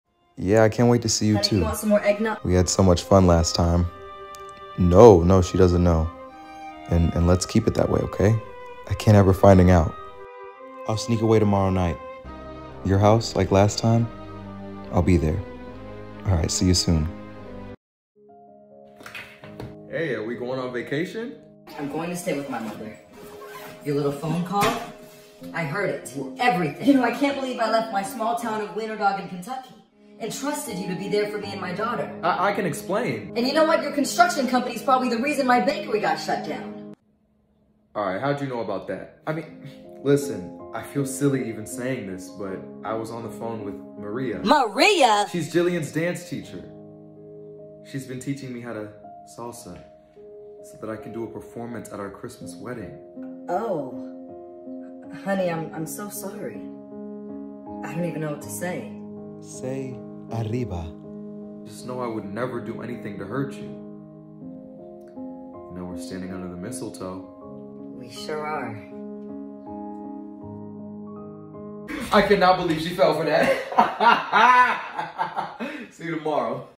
Every Hallmark Christmas Movie Be Like:
She left her small town in Weinerdoggin Kentucky. He's a businessman. Will they survive the holiday after her bakery gets shut down?